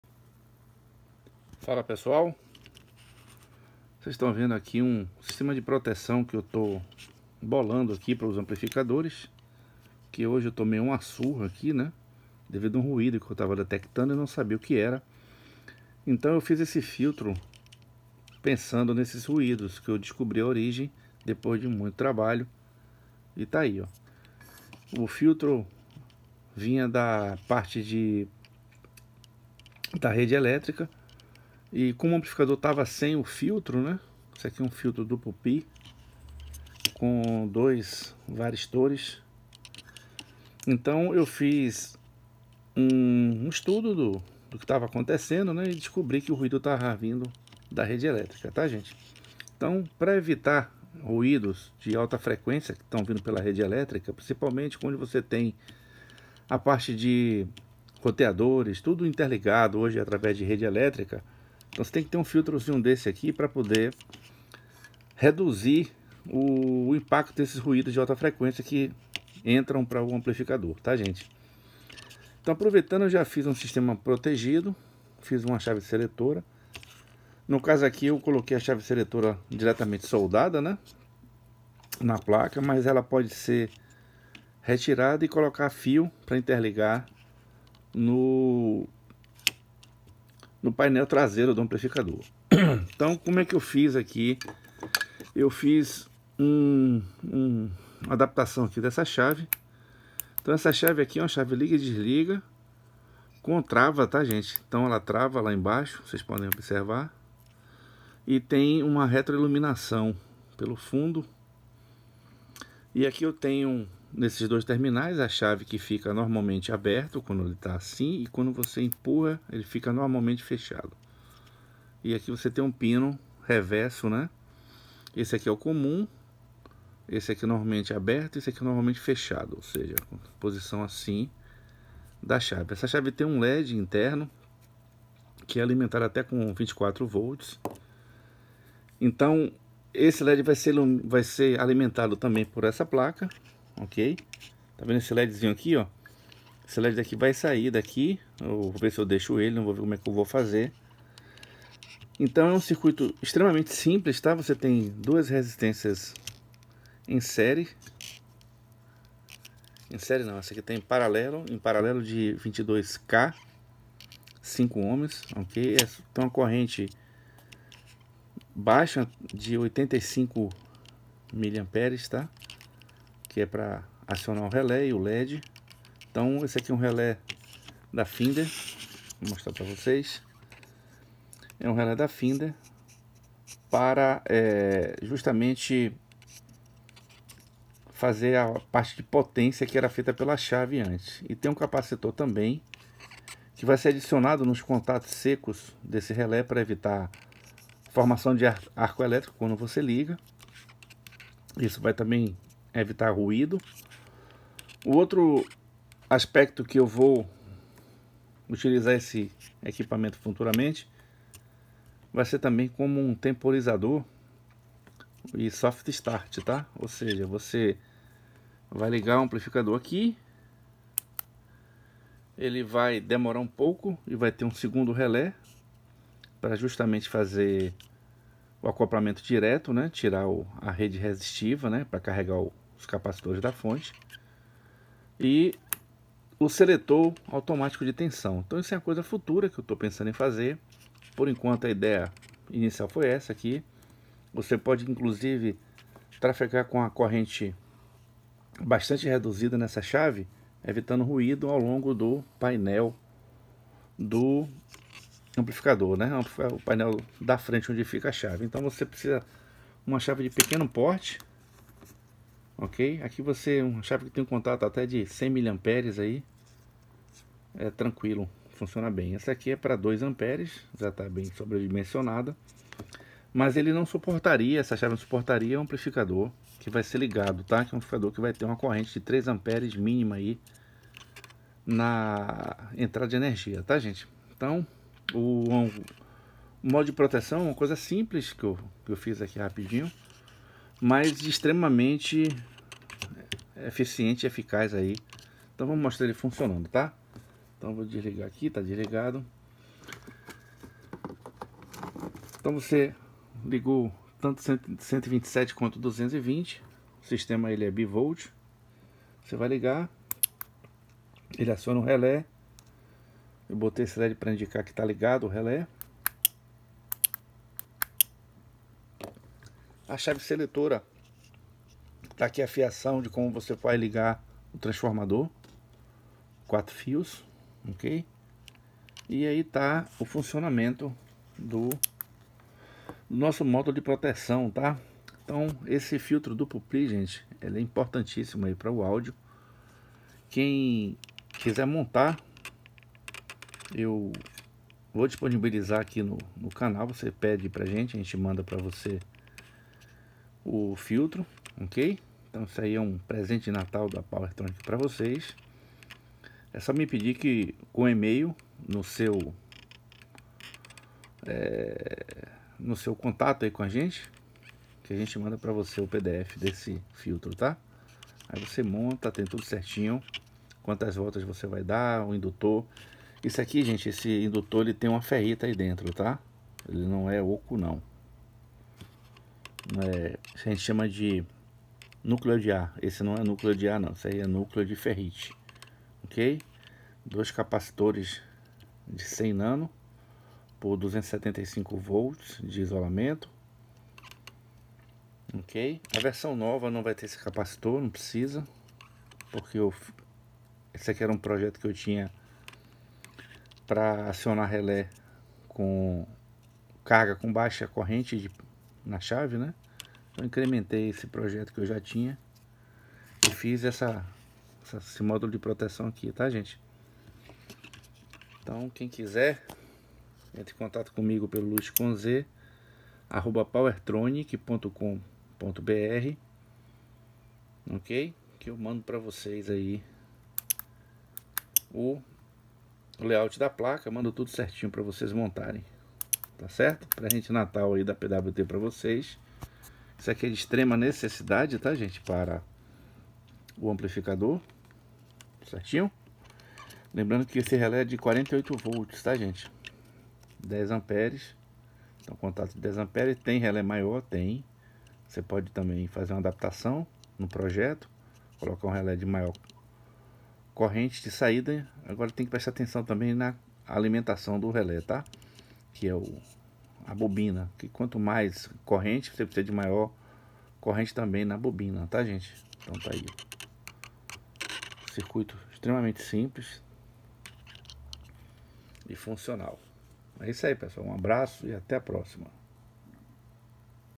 COMO EVITAR RUÍDOS DE RF PRESENTES NA REDE ELÉTRICA.
Neste vídeo uma solução simples e eficaz.